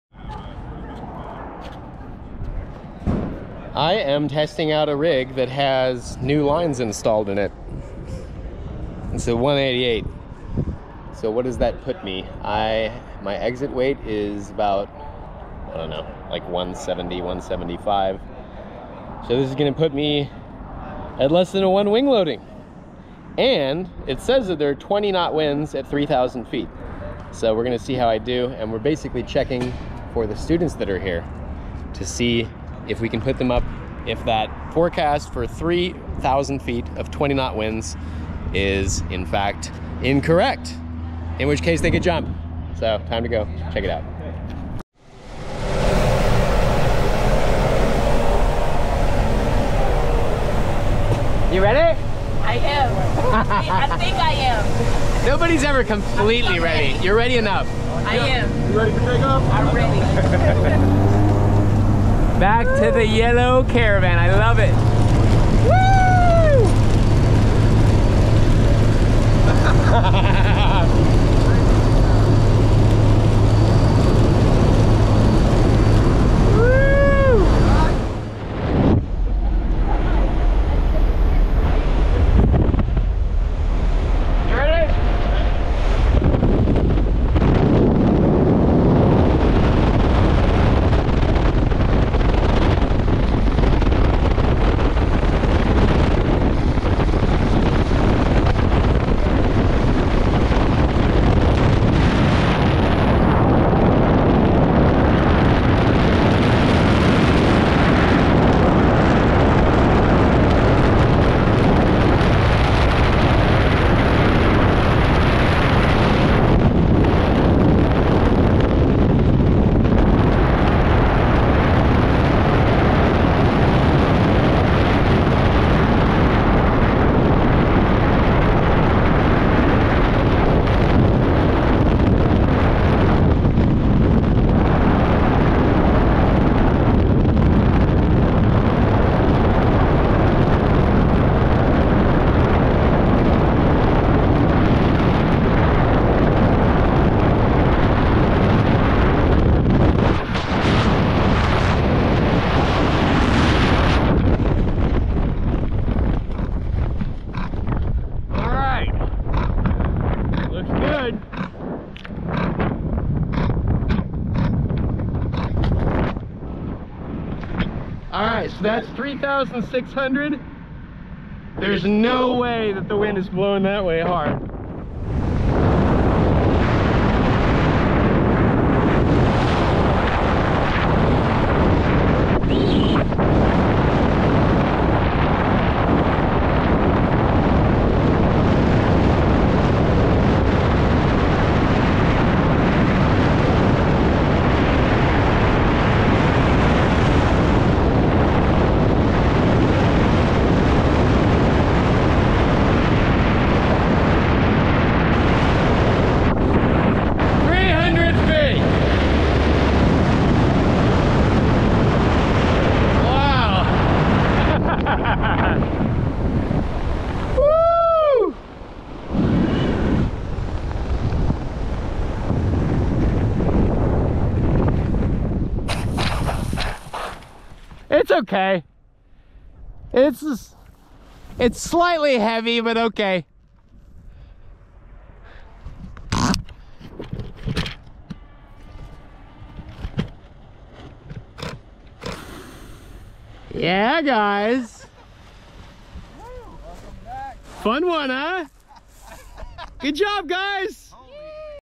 Testing a Pilot 188 w/New Lines
Casey Mongoven tests a dropzone rig to make sure it is good to go for students while also testing upper winds. Skydive Santa Barbara on December 14, 2023.